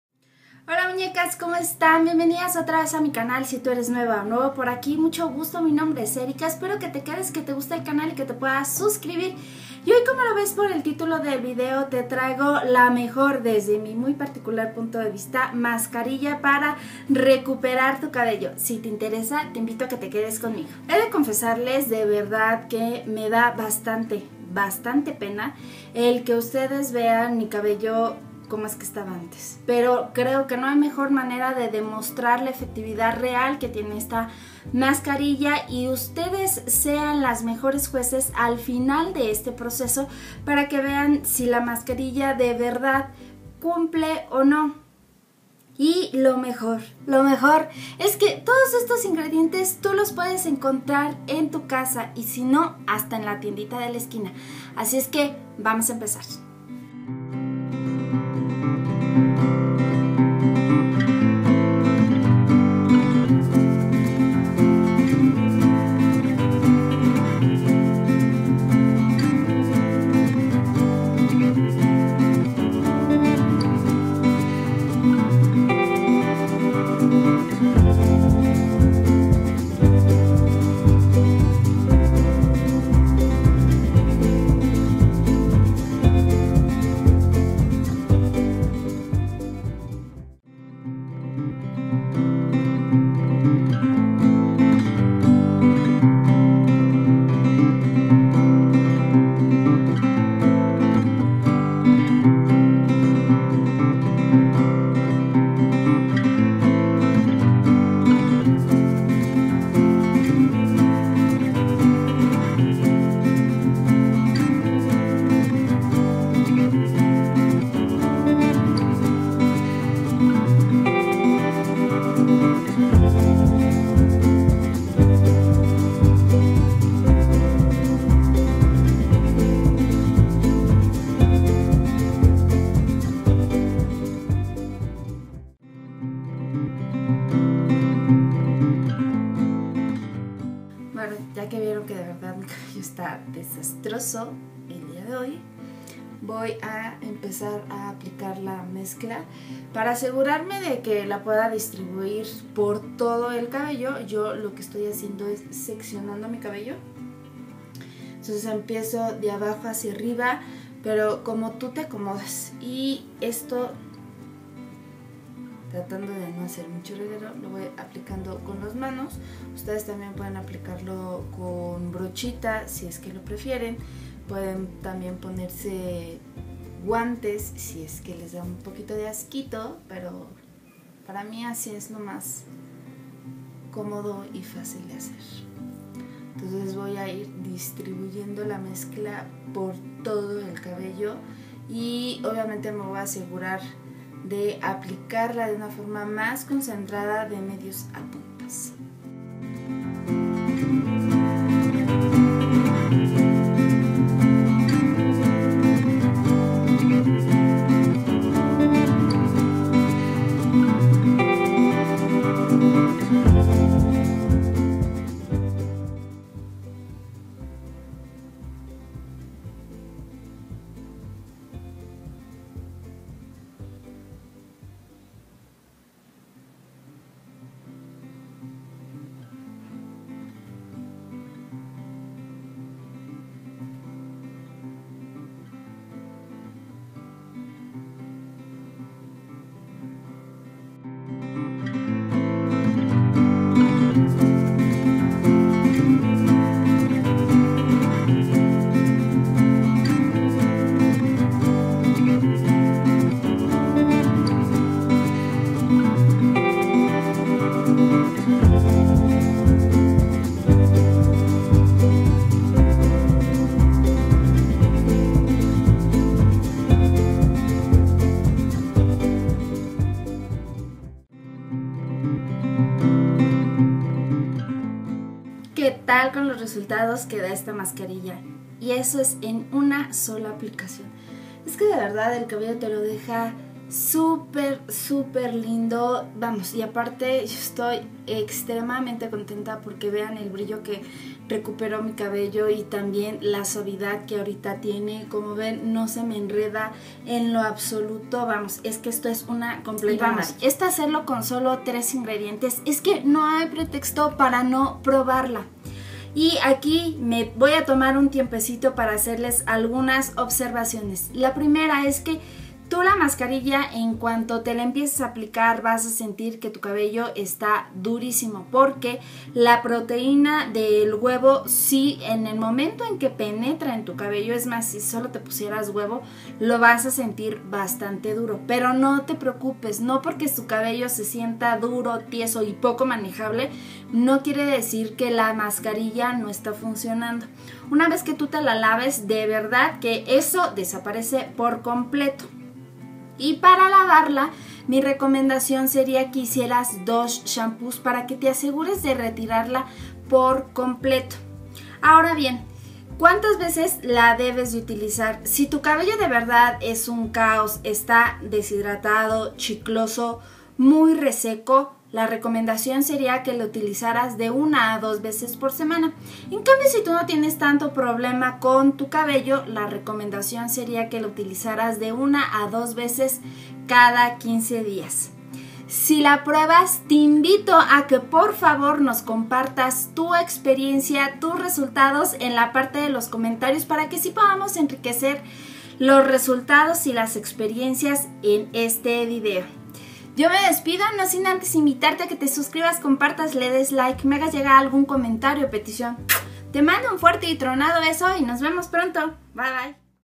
Mascarilla MILAGROSA para el cabello
BIENVENIDAS MUÑECAS!!! Hoy tengo para ustedes la MEJOR mascarilla para recuperar tu cabello, los resultados hablan por sí solos, espero que de verdad se animen a hacerla y nos compartan sus resultados.

Los ingredientes son:

1 1/2 cda. de Maizena o fécula de maíz
1 o 2 huevos (depende del largo de tu cabello)
1 1/2 cda. de Aceite de Olivo

Les mando un fuerte beso y las veo pronto.